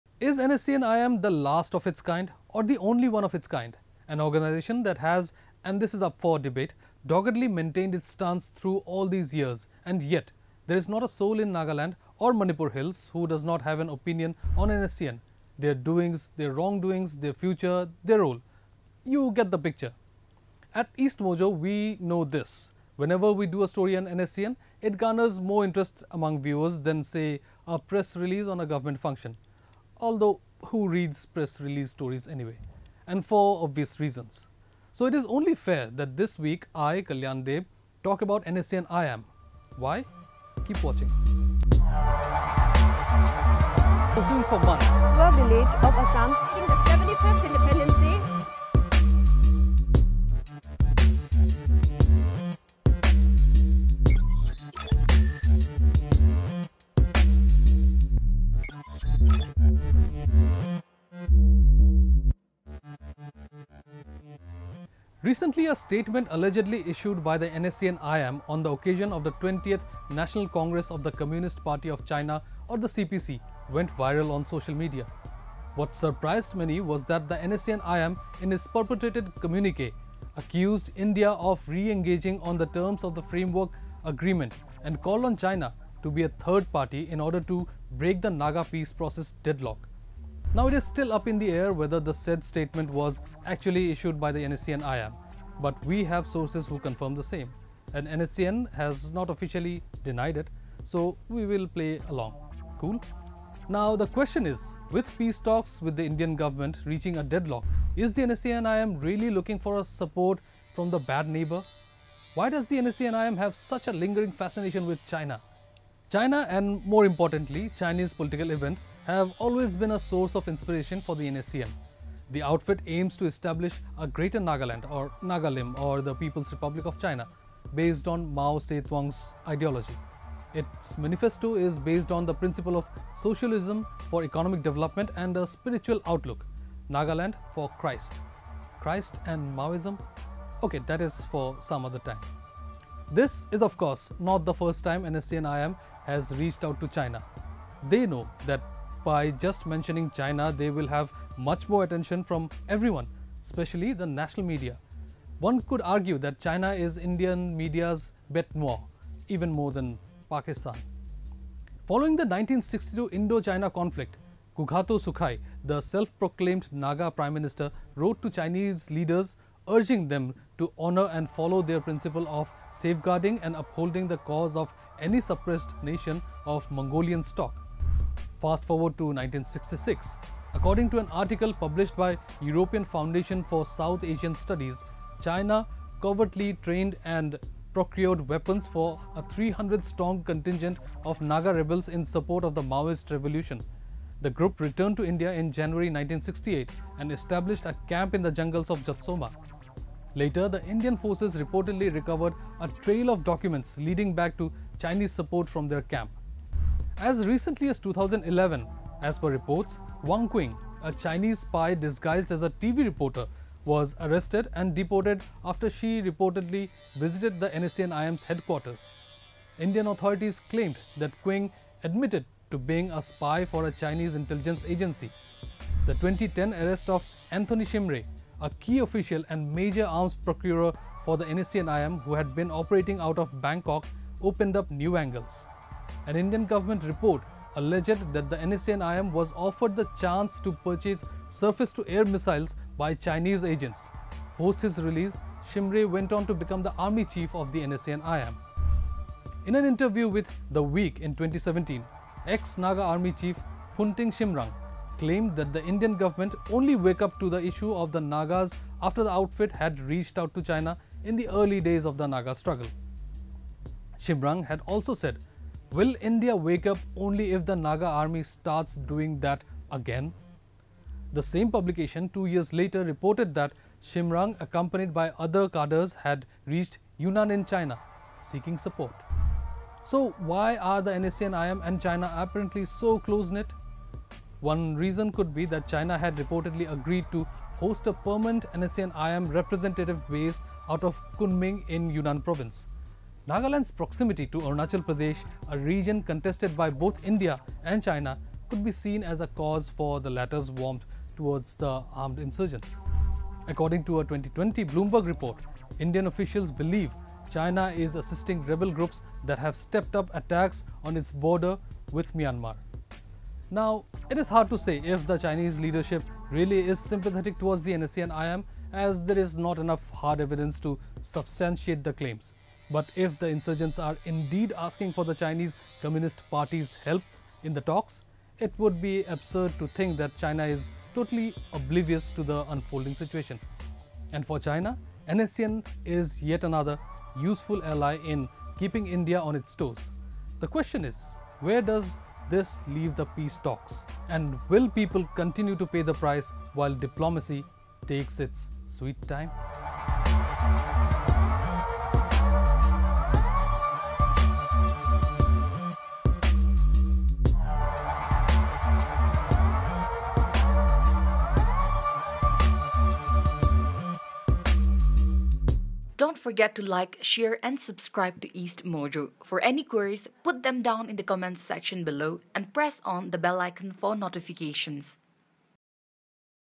Decoded | EP 48 | Is NSCN-IM still connected with China?
China, and more importantly, Chinese political events, have always been a source of inspiration for the National Socialist Council of Nagaland (NSCN). The outfit aims to establish a ‘Greater Nagaland’ or ‘Nagalim’ which is based on Mao Tse Tung’s ideology. Its manifesto is based on the principle of Socialism for economic development and a spiritual outlook – ‘Nagaland for Christ’. Recently, a statement allegedly issued by the NSCN-IM on the occasion of the 20th National Congress of the Communist Party of China (CPC) went viral on social media. What surprised many was that the NSCN-IM, in its purported communique, accused India of reneging on the terms of the Framework Agreement, and called on China to be a “third party” in order to break the Naga peace process deadlock. Now, it is still up in the air whether the said statement was actually issued by the NSCN-IM, but we have sources who confirm it probably was, and NSCN has not officially denied it. Which raises, the question: is the NSCN-IM really looking for China's support?